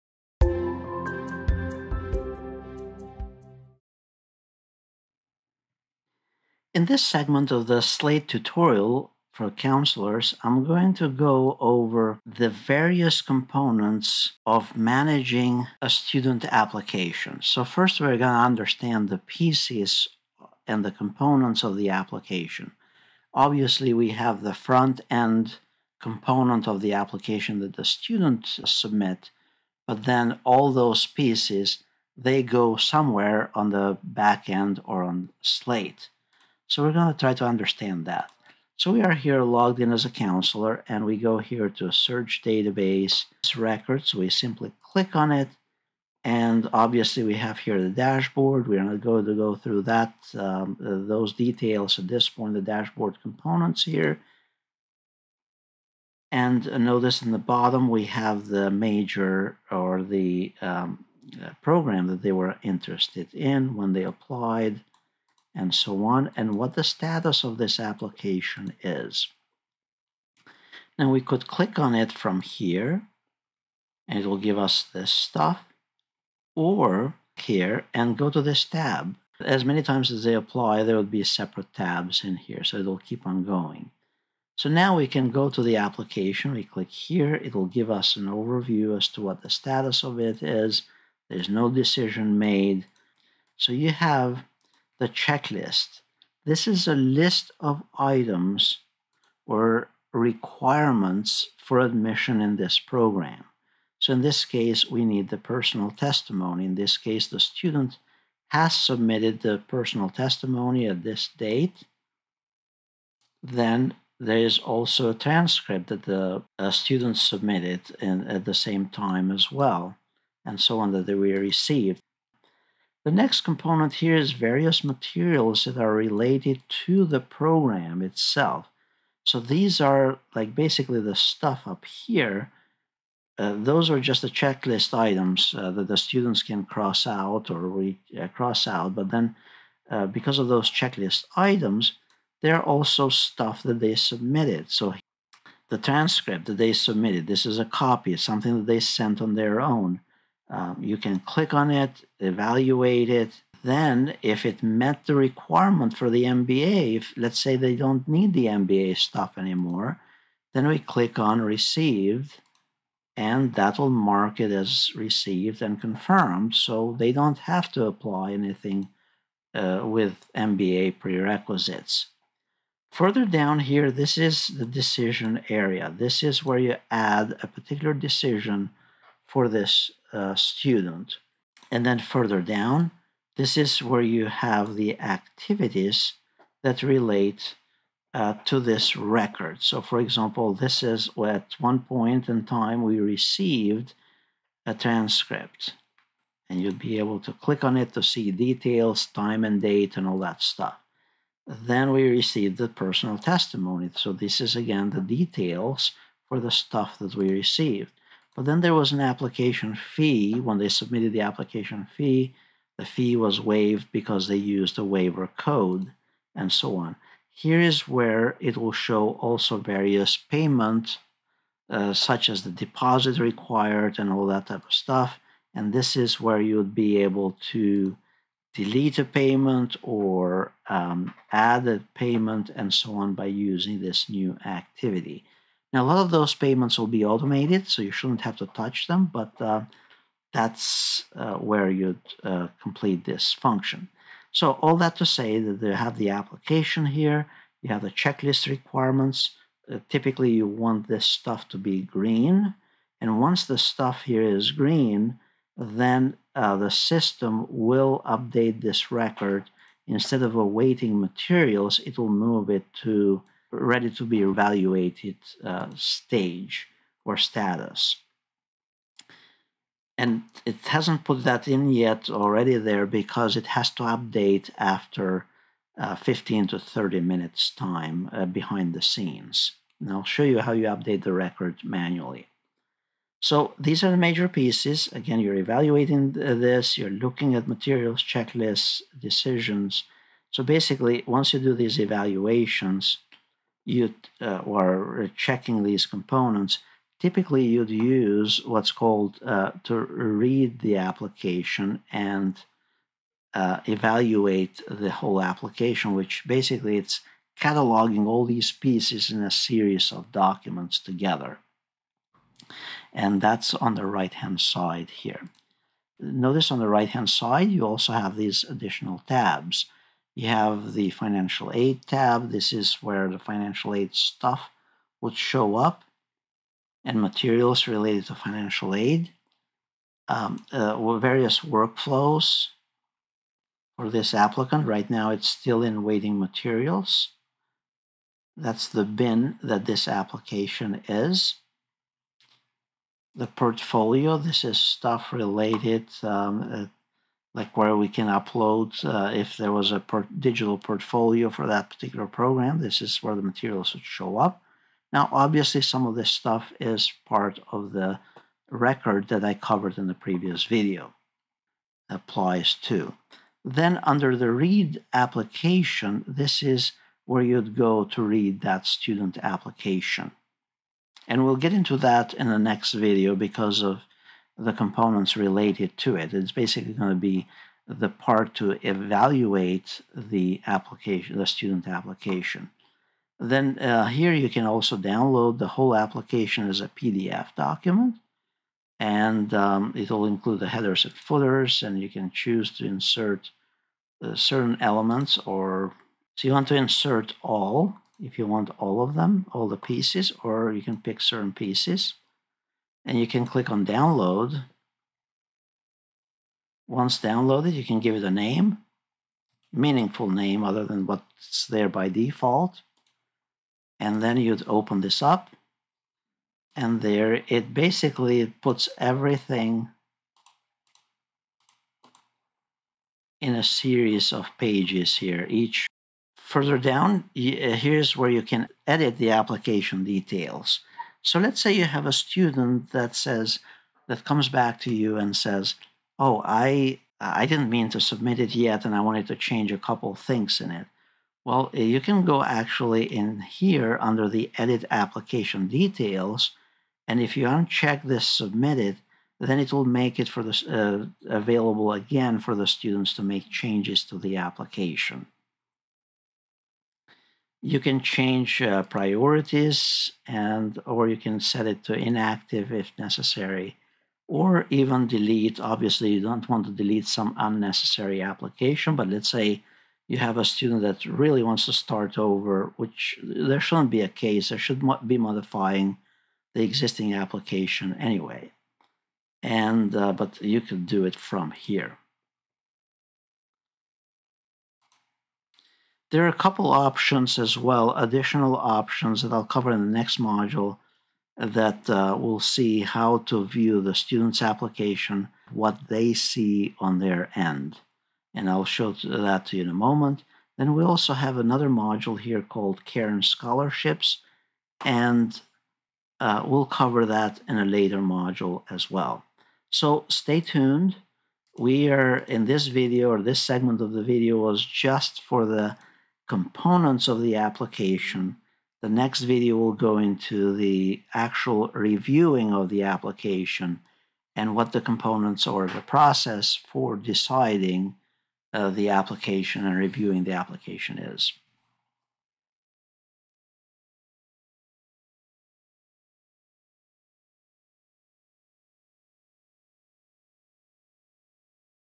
Exploring the Key Components of an Application in Slate by Technolutions (Module 3)
In this comprehensive tutorial, we explore the key components of an application in Slate by Technolutions, the cutting-edge application management system used by colleges and universities worldwide. The complete slate CRM training for counselors is available

We will go over components such as the:
Application tab
Checklists
Materials
Decisions
Activities
Workflows
Accessing the application
and other related areas

➡️ Here are the time markers:
coming soon or send them in.

There are more modules coming including a complete tutorial for the counselors. I would appreciate if you could also press Like as it helps promote the video as useful. New videos will be published on Wednesdays at 11:30am.